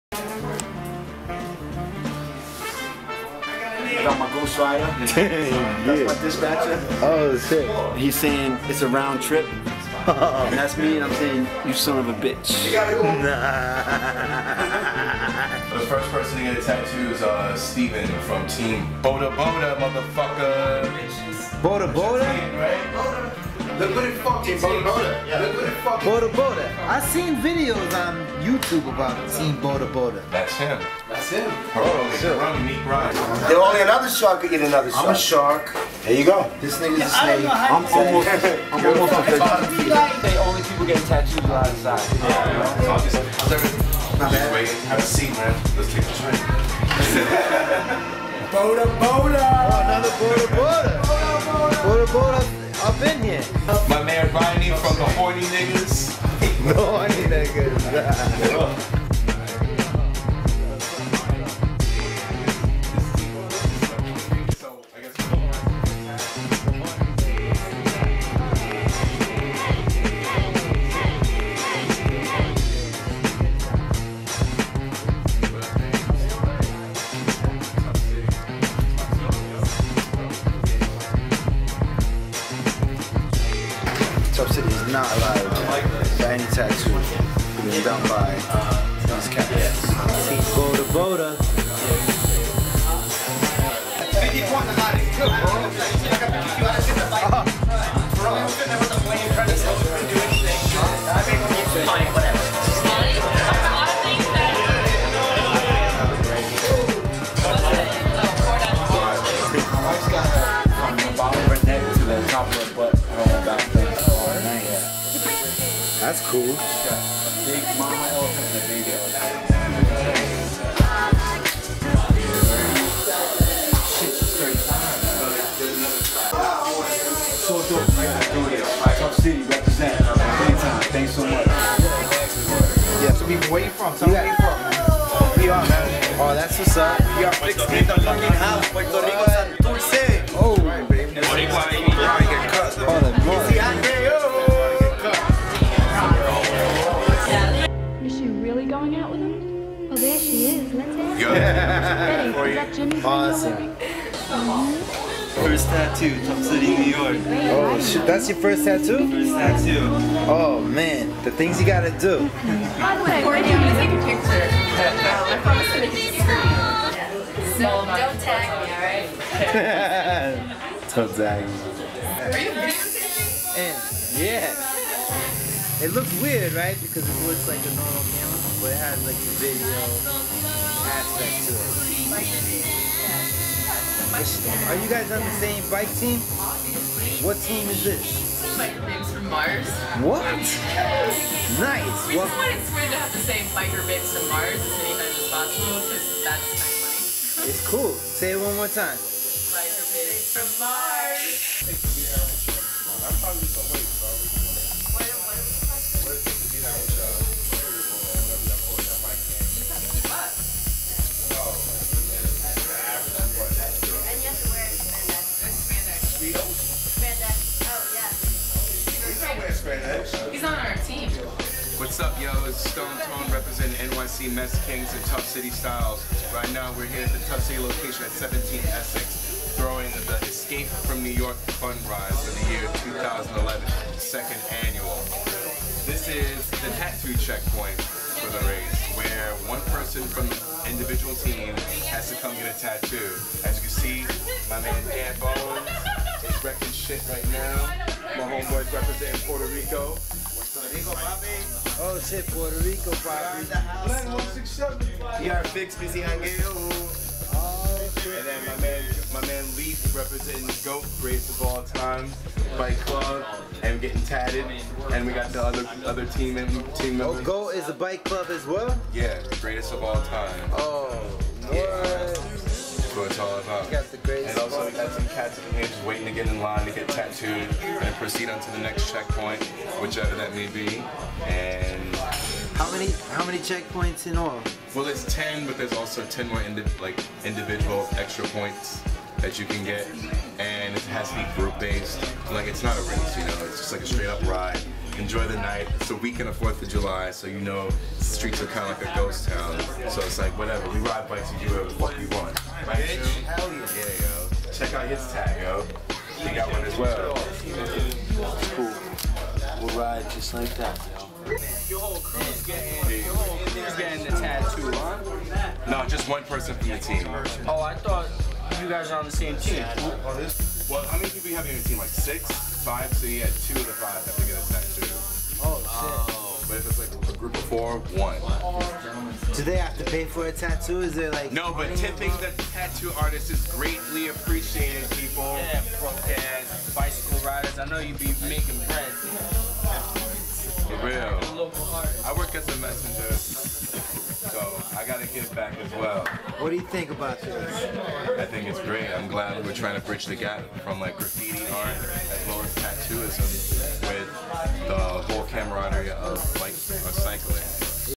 Stoned Tone presents: Tattoo Checkpoint at Tuff City 17 Essex st
ESCAPE FROM NEW YORK Bike Ride 2011
12 teams Started, 10 handed in Manifest so far
SWEATPANTS MONEY MILLIONAIRES ended the race as the Top Team
Video by Tuff City Dwels